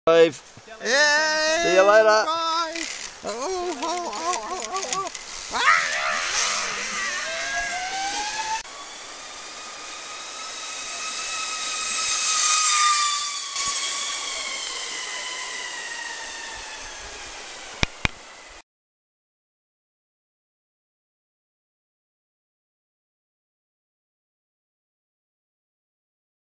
Zip line
Zip line through a beautiful gorge!! one of the options you can choose from in our tour